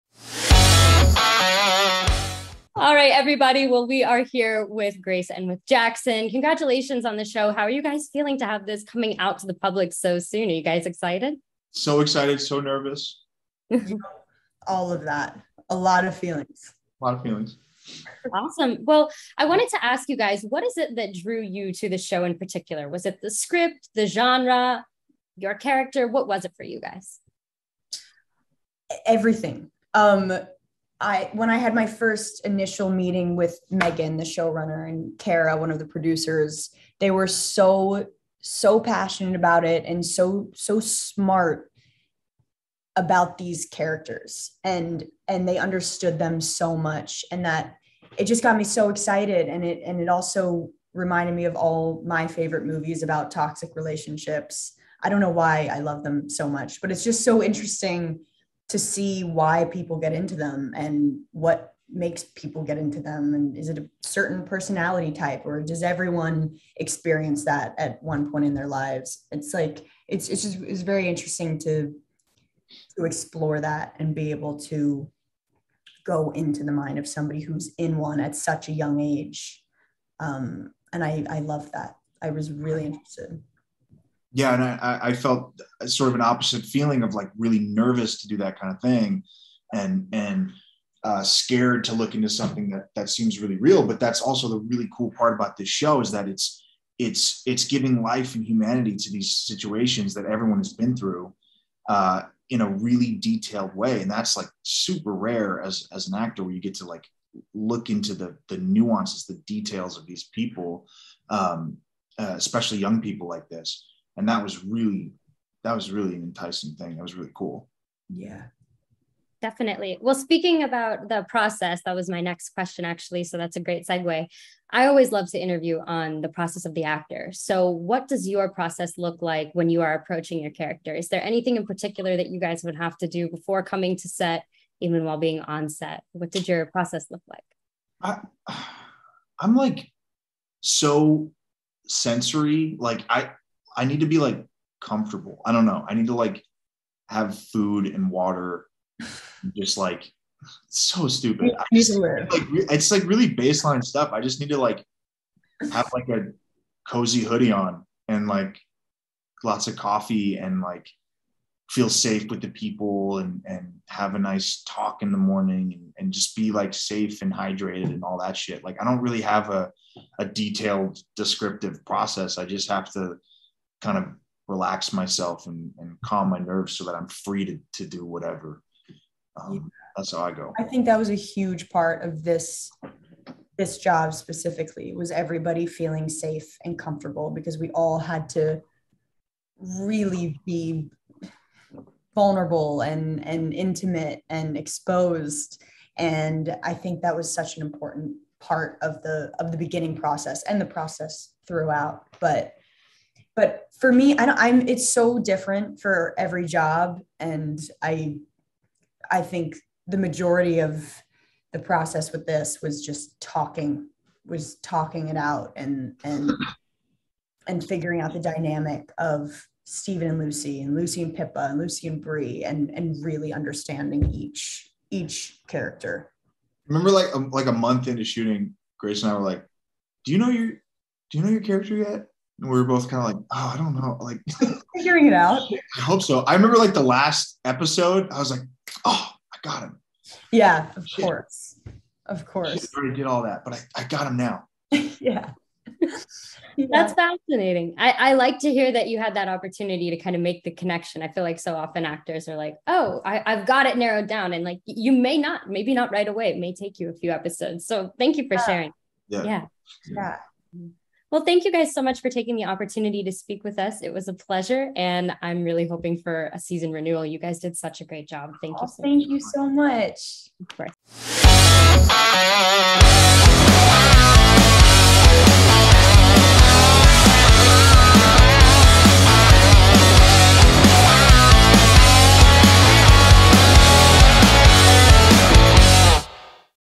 Grace Van Patten and Jackson White Talk Hulu's Tell Me Lies
NEWS x MUSE Hannah Fletcher recently talked with the stars of Hulu's Tell Me Lies, Grace Van Patten and Jackson White.

“Tell Me Lies” follows a tumultuous but intoxicating relationship as it unfolds over the course of 8 years. When Lucy Albright and Stephen DeMarco meet at college, they are at that formative age when seemingly mundane choices lead the way to irrevocable consequences. Although their relationship begins like any typical campus romance, they quickly fall into an addictive entanglement that will permanently alter not only their lives, but the lives of everyone around them.

The series stars Grace Van Patten, Jackson White, Branden Cook, Sonia Mena, Catherine Missal, Benjamin Wadsworth, and Alicia Crowder. Watch it now on Hulu by following the

Host:
Hannah Fletcher